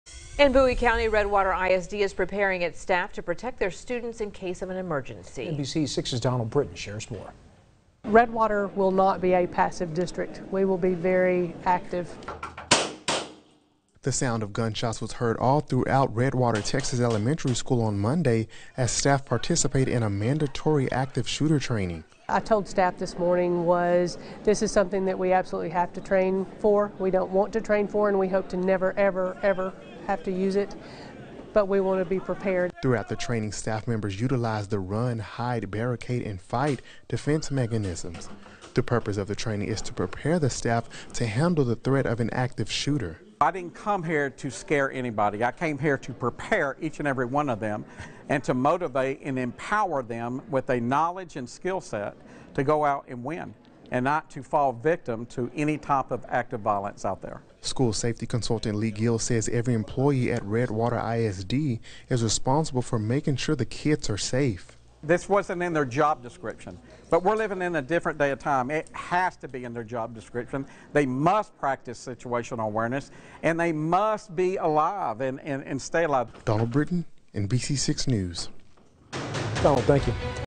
Redwater ISD staff prepares for worst-case scenario with active shooter training
The sound of gunshots was heard throughout Redwater, Texas Elementary School Monday as staff participated in mandatory active shooter training.